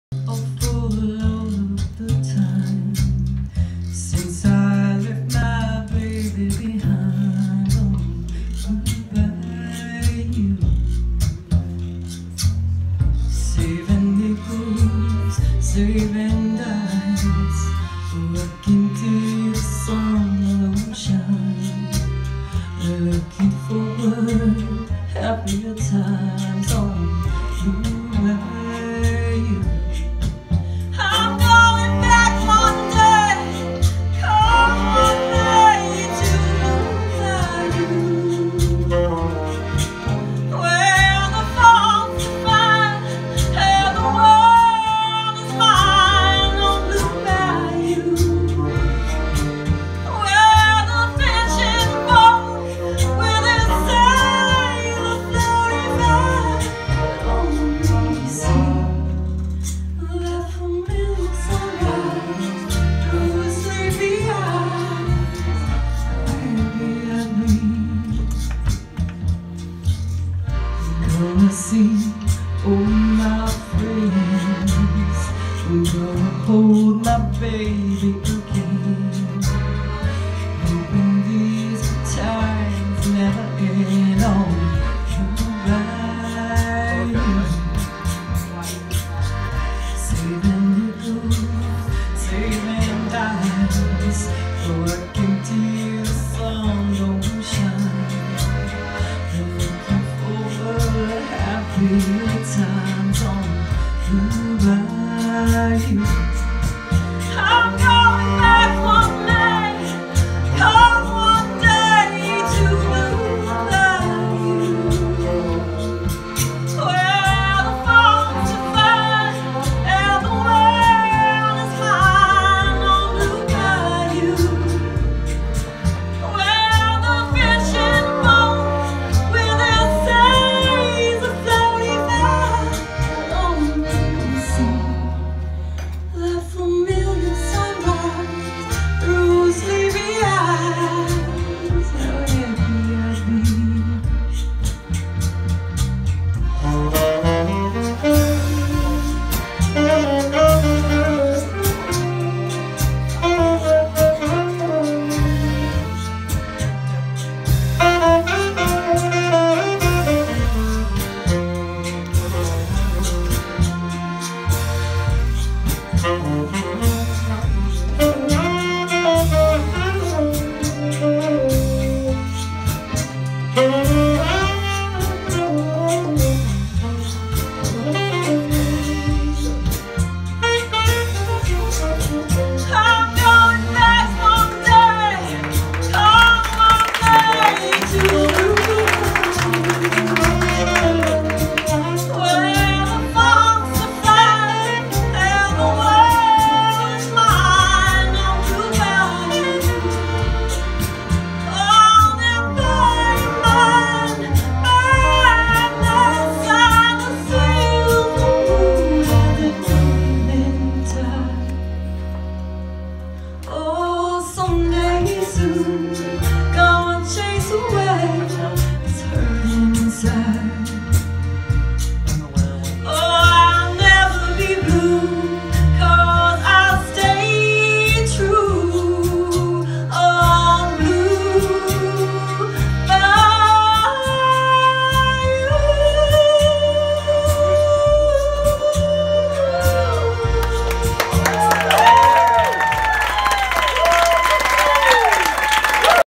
Sidecar Social Club
At Magnolia Roots Music Lounge Wake Forest NC July 27 2019